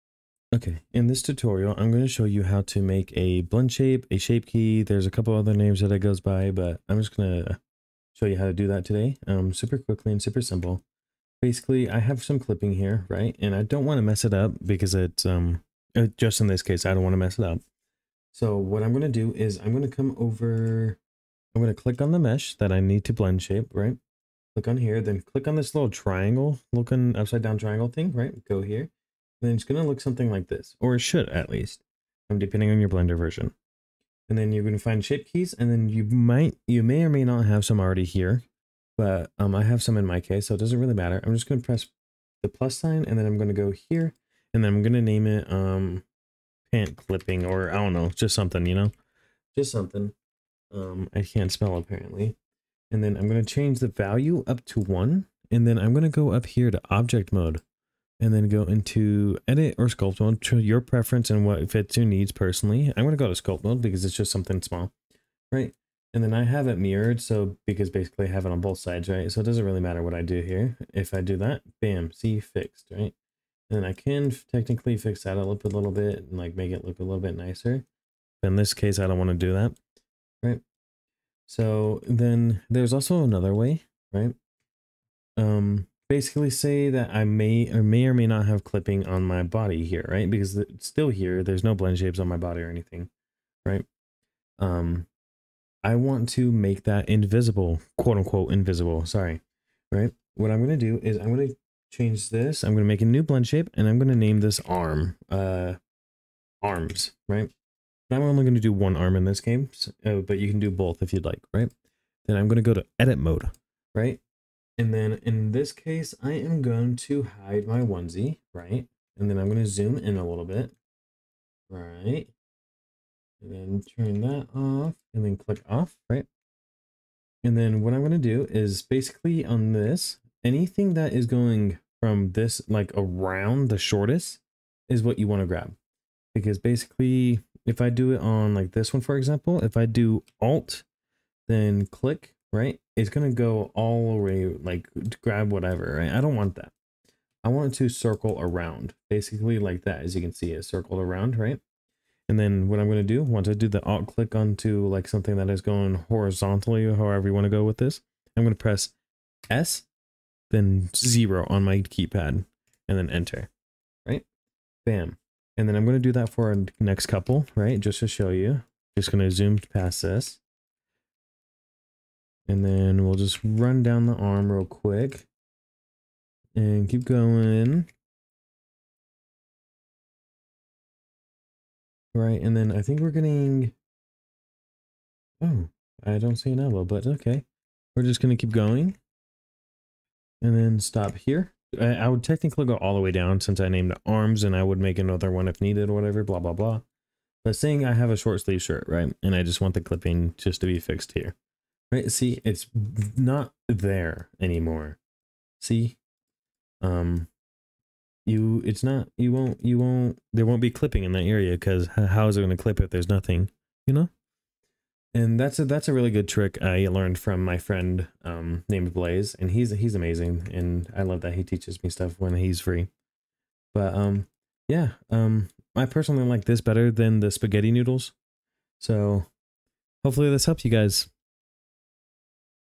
How to: do a blend/shape key in under 5 minutes in Blender - VRChat 3.0 Avatars
This video aims to guide you guys to adding blend/shape keys a lil different and hiding them better. Hopefully this video cleared some of the questions in the air.